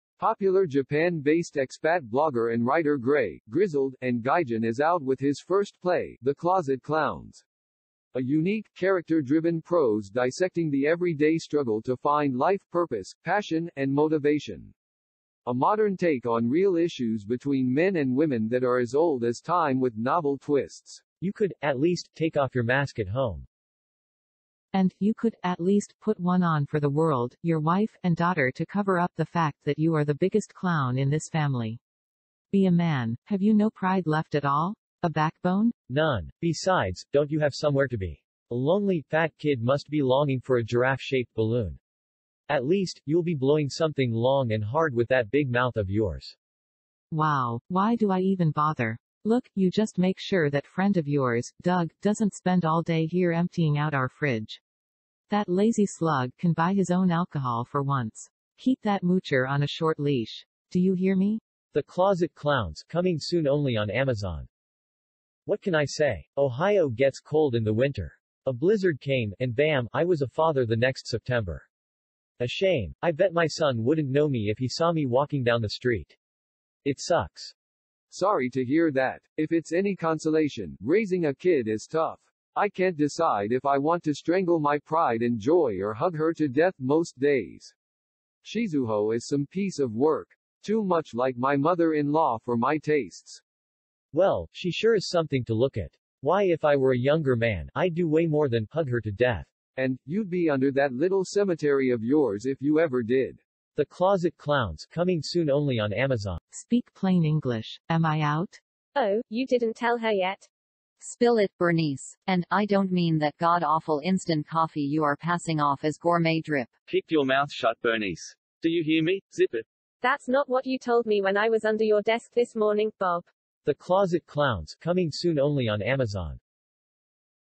"The Closet Clowns"- Play Promotion-001
"The Closet Clowns"

Popular Japan-based blogger and writer Grey, Grizzled, and Gaijin is out with his first play.

A unique, character-driven prose dissecting the every day struggle to find life purpose, passion, and motivation.

A modern take on real issues between men and women that are as old as time with novel twists.